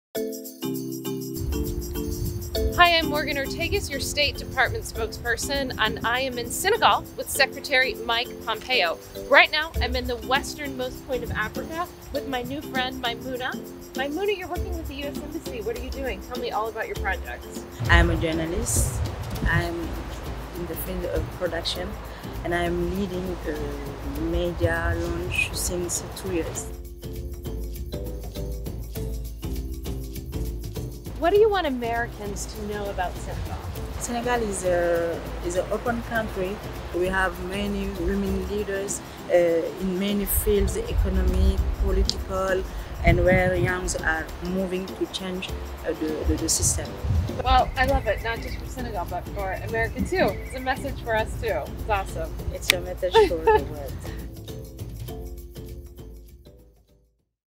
Spox Vox: Spox Talks to Successful TV Owner
Spokesperson Ortagus: What a wonderful visit to the Westernmost Point of Africa with Maimouna Faye, owner of 7TV and 2010 YALI @WashFellowship alumna! Maimouna's success clearly demonstrates the positive impact of @ECAatState programs on Senegal’s future leaders and women’s economic empowerment.